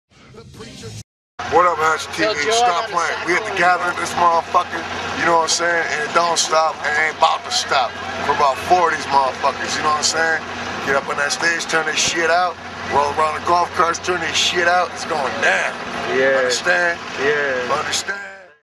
Shaggy 2 Dope unmasked
Necromaster ran into Shaggy at the 4th GOTJ.